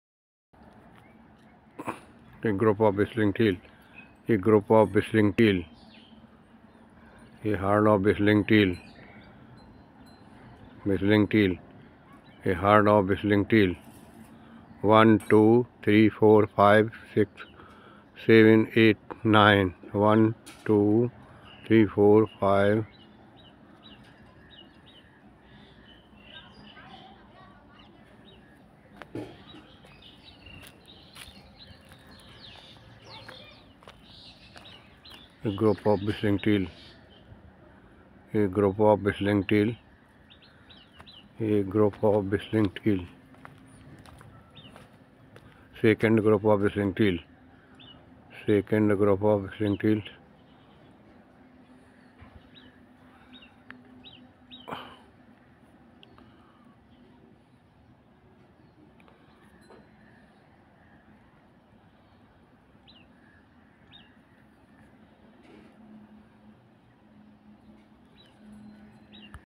WISTLING TEALS nat trend @S B Mishra Dept.Ranger Forest Wild Life UP.
A Group of Wisting teal in fog swimming in Pond Water body Morning scene which is very intersting and lovely scene of Whistling TEALS birds .